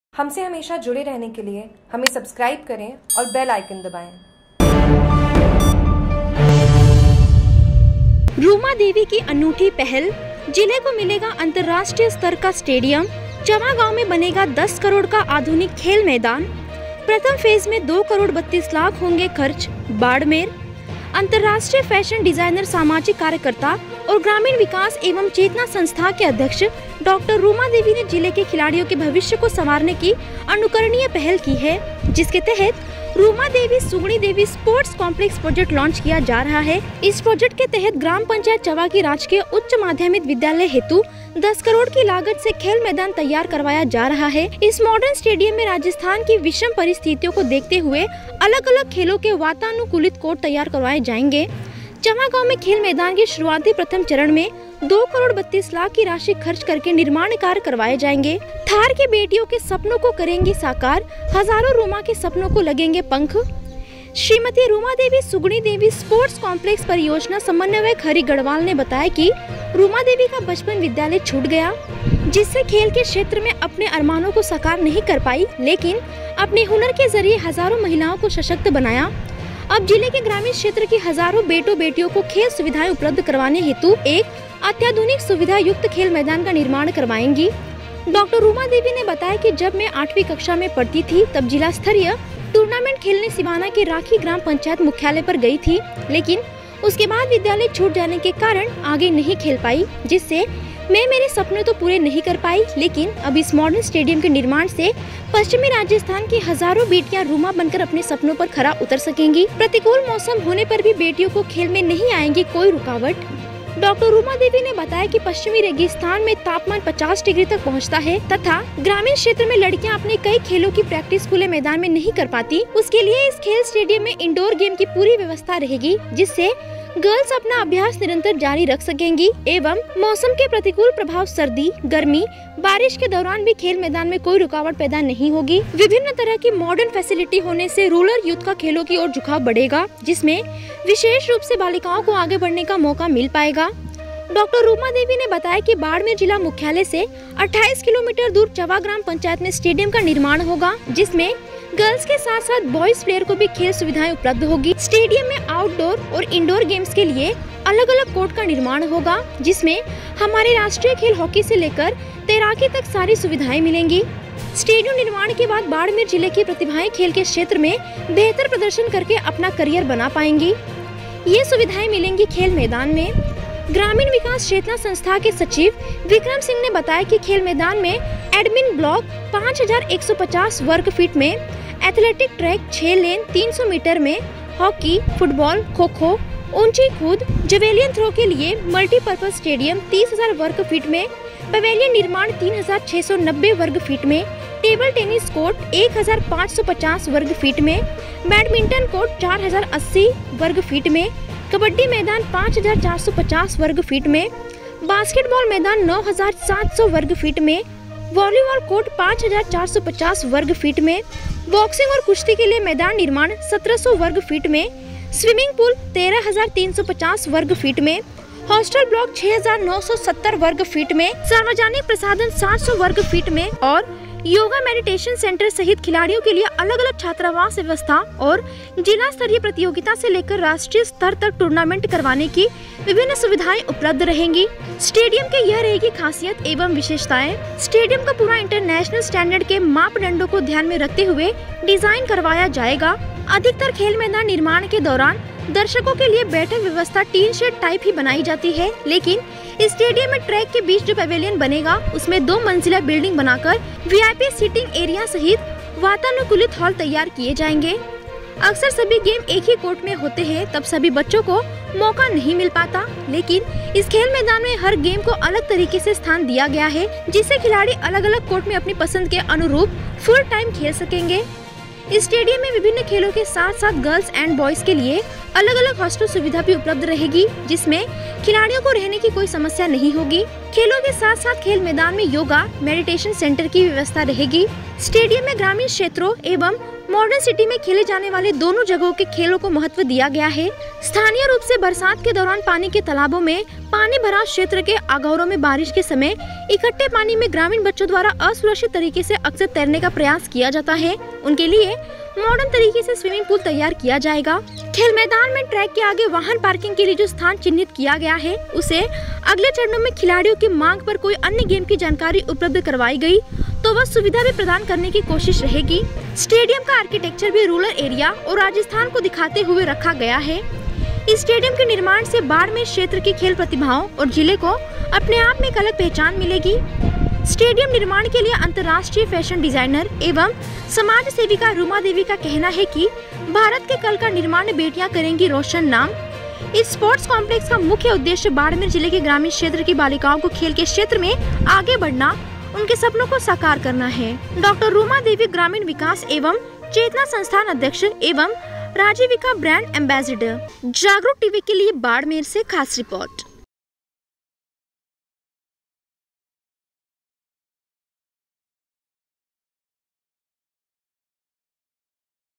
बाड़मेर: जिले को मिलेगा अंतरराष्ट्रीय स्तर का स्टेडियम, चवा गांव में बनेगा दस करोङ का खेल मैदान
रूमा देवी की अनूठी पहल -
जिले को मिलेगा अंतरराष्ट्रीय स्तर का स्टेडियम - चवा गांव में बनेगा दस करोङ का आधुनिक खेल मैदान

 प्रथम फेज में 2 करोड़ 32 लाख होंगे खर्च

बाङमेर- अंतरराष्ट्रीय फैशन डिज़ाइनर, सामाजिक कार्यकर्ता और ग्रामीण विकास एवं चेतना संस्थान की अध्यक्ष डॉ़ रूमा देवी ने जिले के खिलाड़ियों के भविष्य को संवारने की अनुकरणीय पहल  की है।
जिसके तहत रूमादेवी- सुगणी देवी स्पोर्ट्स काम्प्लेक्स प्रोजेक्ट लांच किया जा रहा है। इस प्रोजेक्ट के तहत ग्राम पंचायत चवा की राजकीय उच्च माध्यमिक विद्यालय हेतु 10 करोङ की लागत से खैल मैदान तैयार करवाया जा रहा है, इस माँडर्न स्टेडियम में राजस्थान की विषम परिस्थितियों को देखते हुए अलग-अलग खेलों के वातानुकूलित कोर्ट तैयार करवाए जाएंगे।
चवा गांव में खेल मैदान के शुरुआती प्रथम चरण में 2 करोङ 32 लाख की राशि खर्च करके निर्माण कार्य करवाए जाएंगे।

थार की बेटियों के सपनो को करेगी साकार, हजारों रूमा के सपनो को लगेंगे पंख

श्रीमती रूमादेवी-सुगणी देवी स्पोर्ट्स काम्प्लैक्स परियोजना समन्वयक हरि गढवाल ने बताया कि रूमादेवी का बचपन में विधालय छूट गया,जिससे खेल के क्षेत्र में अपने अरमानो को साकार नहीं कर पाई लेकिन अपने हुनर के जरिए हजारों महिलाओं को सशक्त बनाया। अब जिले के ग्रामीण क्षेत्र की हजारो बेटों- बेटियों को खेल सुविधाएं उपलब्ध करवाने हेतु एक अत्याधुनिक सुविधायुक्त खेल मैदान का निर्माण करवाएगी।

डाँ रुमा देवी ने बताया कि जब में आठवी कक्षा में पढती थी तब जिला स्तरीय टूर्नामेंट खेलने सिवाना के राखी ग्राम पंचायत मुख्यालय पर गई थी। लेकिन उसके बाद विद्यालय छूट जाने के कारण आगे नहीं खेल पाई, जिससे में मेरे सपने तो पूरे नहीं कर पाई, लेकिन अब इस माँडर्न स्टेडियम के निर्माण से पश्चिमी राजस्थान की हजारों बेटियां रूमा बनकर अपने सपनों पर खरा उतर सकेगी।

प्रतिकूल मौसम होने पर भी बेटियों को खेल में नहीं आएगी कोई रूकावट
डॉ रूमा देवी ने बताया कि पश्चिमी रेगिस्तान में तापमान 50 डिग्री तक पहुंचता है तथा ग्रामीण क्षेत्र में लड़कियां अपने कई खेलों की प्रैक्टिस खुले मैदान में नहीं कर पाती, उसके लिए इस खेल स्टेडियम में इंडोर गेम की पूरी व्यवस्था रहेगी जिससे गर्ल्स अपना अभ्यास निरंतर जारी रख सकेगी एवं मौसम के प्रतिकूल प्रभाव सर्दी, गर्मी, बारिश के दौरान भी खेल मैदान में कोई रुकावट पैदा नहीं होगी।

विभिन्न तरह की माँडर्न फैसिलिटी होने से रूरल यूथ का खेलों की ओर झुकाव बढेगा,जिसमें विशेष रूप से बालिकाओं को आगे बढ़ने का मौका मिल पायेगा।

-स्टेडियम को पूरा इंटरनेशनल स्टैंडर्ड के मापदंडो को ध्यान में रखते हुए डिजाइन करवाया गया है।

- अधिकतर खेल मैदान निर्माण के दौरान दर्शकों के लिए बैठक व्यवस्था टीनशेड टाइप ही बनाई जाती हैं, लेकिन इस स्टेडियम में ट्रैक के बीच जो पवेलियन बनेगा,उसमे दो मंजिला बिल्डिंग बनाकर वीआईपी सिटिंग एरिया सहित वातानुकूलित हाॅल तैयार किए जाएंगे।

-अक्सर सभी गेम एक ही कोर्ट में होते हैं तब सभी बच्चों को मौका नहीं मिल पाता,लेकिन इस खेल मैदान में हर गेम को एक अलग तरीके से स्थान दिया गया है,जिससे खिलाड़ी अलग-अलग कोर्ट में अपनी पसन्द के अनुरूप फुल टाइम खेल सकेंगे।

-इस स्टेडियम में विभिन्न खेलों के साथ-साथ गर्ल्स एंड बॉयज के लिए अलग-अलग हॉस्टल सुविधा भी उपलब्ध रहेगी, जिससे खिलाड़ीयो को रहने की कोई समस्या नहीं होगी।

- खेलों के साथ-साथ खेल मैदान में योगा, मेडिटेशन सेंटर की भी व्यवस्था रहेगी।

- स्टेडियम में ग्रामीण क्षेत्रों एवं मॉर्डन सिटी में खेले जाने वाले दोनों जगह के खेलों को महत्व दिया गया है।

- स्थानीय रूप से बरसात के दौरान पानी के तालाबो में, पानी भराव क्षेत्र के आगौरो में बारिश के समय इकट्ठे पानी में ग्रामीण बच्चों द्वारा असुरक्षित तरीके से अक्सर तैरने का प्रयास किया जाता है, उनके लिए मॉडर्न तरीके से स्विमिंग पूल तैयार किया जाएगा।

-खेल मैदान में ट्रैक के आगे वाहन पार्किंग के लिए जो स्थान चिन्हित किया गया है,उसे अगले चरणों में खिलाडियों की मांग पर कोई अन्य गेम की जानकारी उपलब्ध करवाई गई तो वह सुविधा भी प्रदान करने की कोशिश रहेगी।

-स्टेडियम का आर्किटेक्चर भी  रूरल एरिया और राजस्थान को दिखाते हुए रखा गया है।

-इस स्टेडियम के निर्माण से बाङमेर क्षेत्र की खेल प्रतिभाओं और जिले को अपने आप में एक अलग पहचान मिलेगी।

स्टेडियम निर्माण के लिए अंतरराष्ट्रीय फैशन डिज़ाइनर एवं समाजसेविका रुमादेवी का कहना है की - "भारत के कल का निर्माण-बेटियां करेगी रोशन नाम"। इस स्पोर्ट्स काम्प्लैक्स का मुख्य उद्देश्य बाङमेर जिले के ग्रामीण क्षेत्र की बालिकाओं को खेल के क्षेत्र में आगे बढ़ाना, उनके सपनों को साकार करना है।
(डॉ. रूमा देवी,ग्रामीण विकास एवं चेतना संस्थान अध्यक्ष एवं राजीविका ब्रांड एंबेसडर)